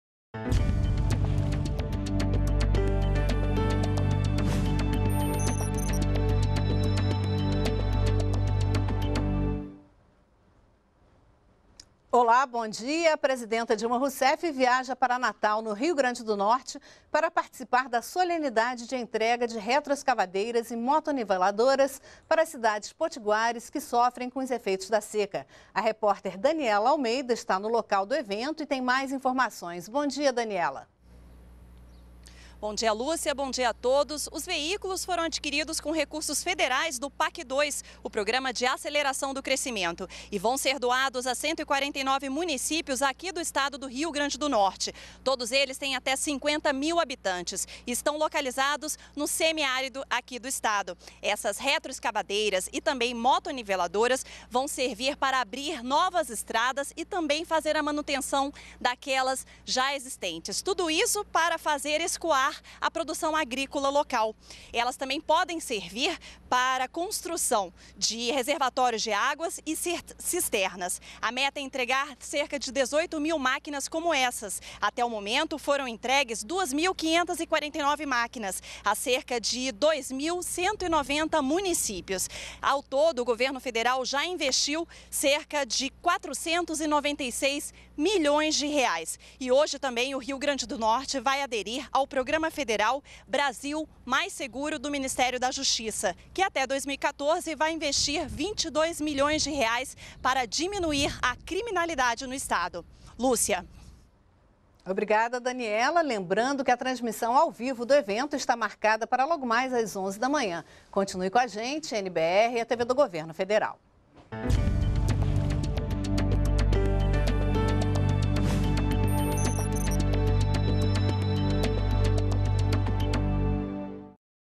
Cidades do Rio Grande do Norte recebem máquinas retroescavadeiras e motoniveladoras
ATIVIDADES DA PRESIDENTA -- 03.06.13: Nesta segunda-feira (3), 149 municípios com até 50 mil habitantes do Rio Grande do Norte serão beneficiados com máquinas retroescavadeiras e motoniveladoras. O maquinário será entregue pela presidenta Dilma Rousseff para cidades do semiárido. Os equipamentos vão contribuir para a construção e manutenção de estradas vicinais e na construção de cisternas e reservatórios de água. A entrega faz parte do Programa de Aceleração do Crescimento (PAC 2). Até o momento, já foram entregues 2.549 máquinas que já beneficiam 2.190 municípios. Na mesma ocasião, o governo do estado vai aderir ao programa "Brasil mais Seguro" do Ministério da Justiça para diminuir a criminalidade na região.